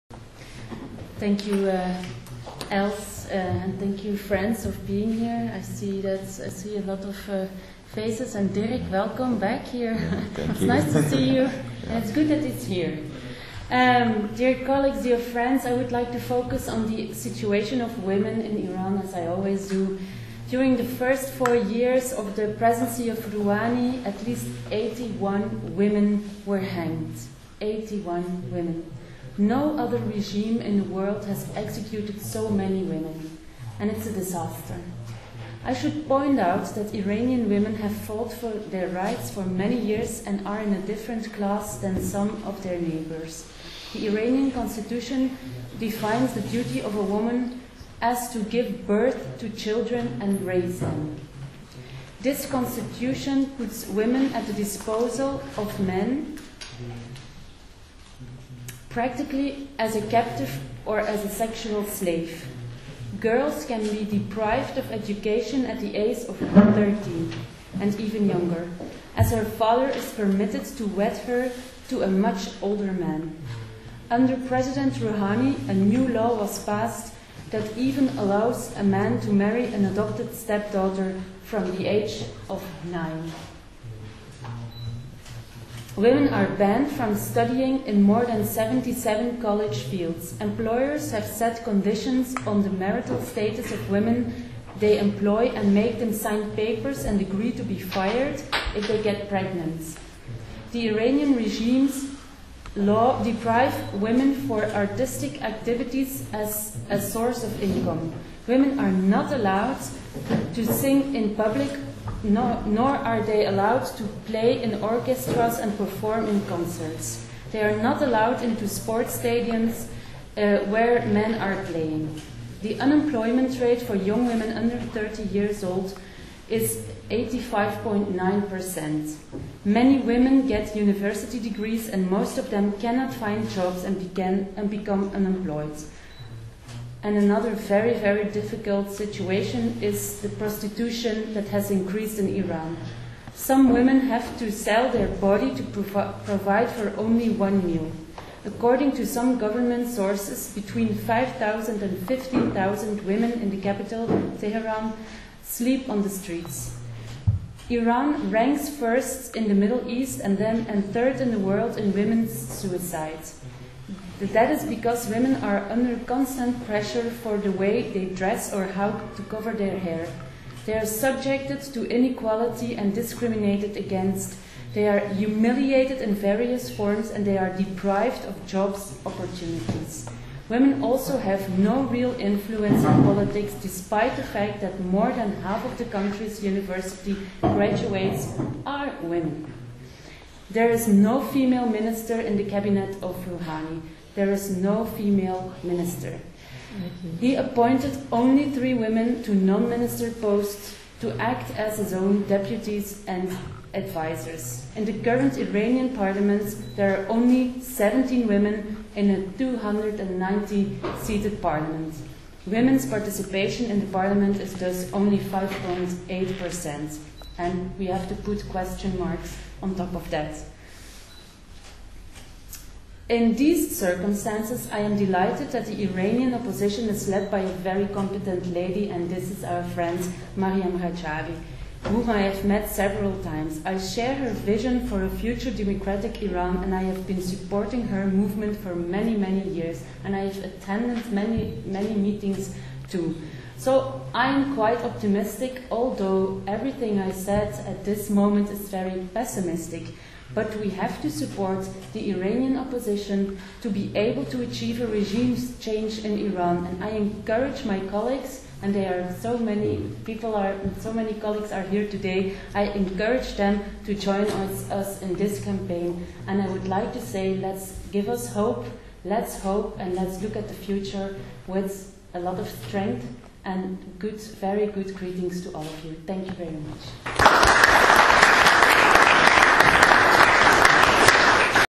Speech by Belgian MP Nele Lijnen on Human Rights in Iran, Belgian Parliament, Brussels 17 Oct 2017
Belgian Lawmakers urge European governments to conditions relations with Iran to a halt to executions and progress on human rights
On Tuesday 17 October 2017, the Belgian Committee of Parliamentarians and Mayors for a Democratic Iran, held a meeting in the Belgian Federal Parliament on the situation of human rights in Iran.
Several members of parliament and senators from different political parties spoke in this meeting which was chaired by Ms Els Van Hoof, member of the Foreign Affairs Committee of Belgian Parliament from the Christian Democratic Party.
The Belgian lawmakers strongly condemned the human rights violations during the first term of Hassan Rouhani and called on the European countries to condition their relations with Iran to a suspension of executions and a clear progress on human rights and the situation of women.
They emphasized that Rouhani has defended the executions and has expressed his support for the Islamic Revolutionary Guard Corps (IRGC) which controls most of the Iranian economy, has been the main tool of repression inside Iran and has recently been black-listed by the US government for its terrorist activities in the region. His foreign minister, Javad Zarif who is regarded as a so called “moderate”, expressed his full support last week for the Revolutionary Guards. MPs called on the EU to also black-list the IRGC.
Belgian politicians expressed their support for the Iranian democratic opposition, led by Mrs Maryam Rajavi and emphasized that her campaign to seek justice for the victims of the 1988 massacre of 30,000 political prisoners, mostly from the opposition PMOI/MEK, has shaken Iran and has now become an international issue. They called for an independent investigation to prosecute those responsible.
Rouhani replaced his justice minister who was member of the ‘death committee’ in Tehran during the 1988 massacre with Alireza Avai. The new minister Avai ,who was member of the death committee in southern Iran, is also on the EU black list for human rights violations since 2011.
So with those kind of figures in the cabinet of this “moderate” president, we can firmly say that this is the same regime as 30 years ago, only with a different appearance that is more suitable for the west to deal with.
The speakers expressed their deep disappointment of the current attitude of the EU and especially the High Representative Federica Mogherini as she ignores repression against women in Iran and even went to Tehran to take part in the inauguration ceremony of Rouhani. Such a policy of closing the eyes to human rights violations and the repression of women in Iran is an embarrassment and cannot be justified because EU wants to do business with Iran. This silence by EU’s foreign policy chief, is extremely damaging for the reputation of Europe and is in the benefit of the theocratic state of the mullahs who get encouraged to carry on their repressive rule.
We as Belgian politicians should remind our government to put human rights first and that this regime in Tehran is not a democratically elected government. It is a religious dictatorship. So we should at least insist that any expansion of relationship with this regime must be conditioned to a suspension of executions and a clear progress on human rights and situation of women.